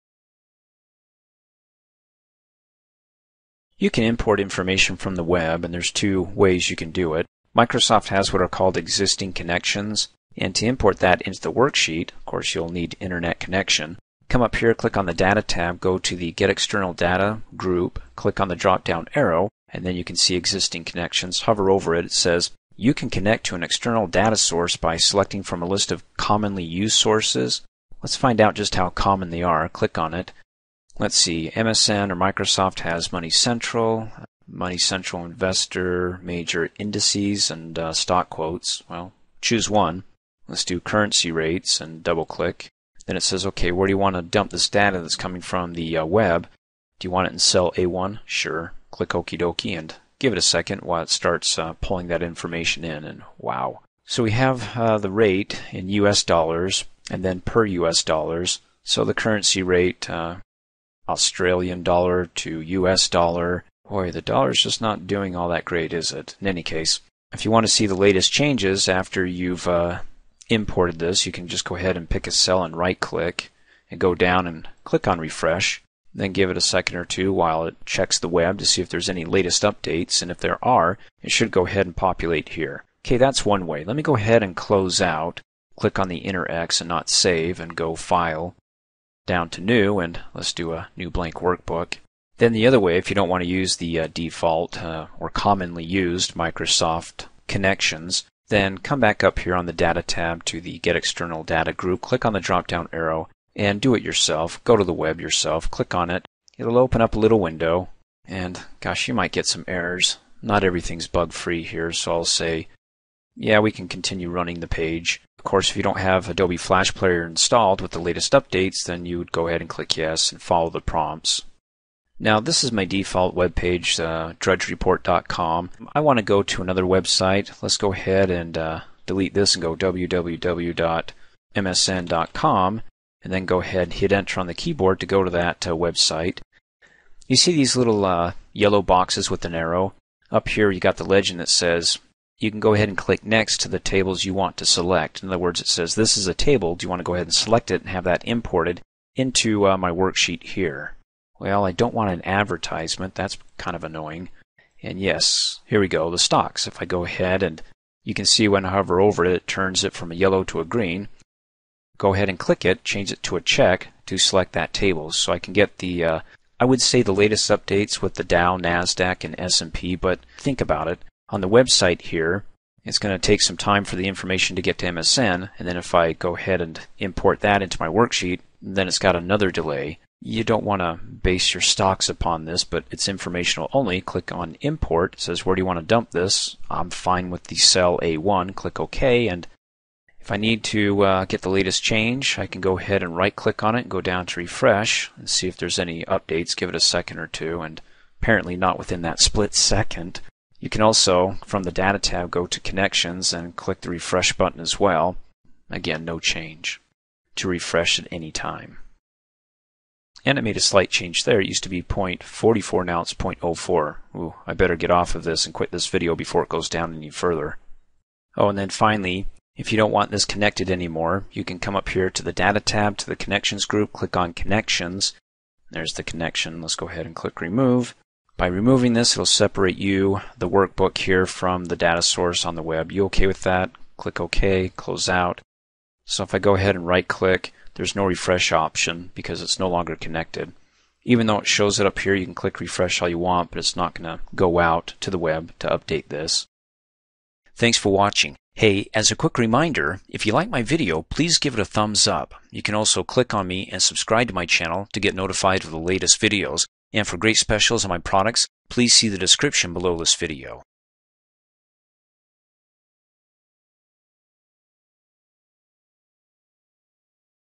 Microsoft Excel 2010: Import Data from Websites
Microsoft Excel 2010 training video on how to import data from a web page and refresh it to get the website's latest updates.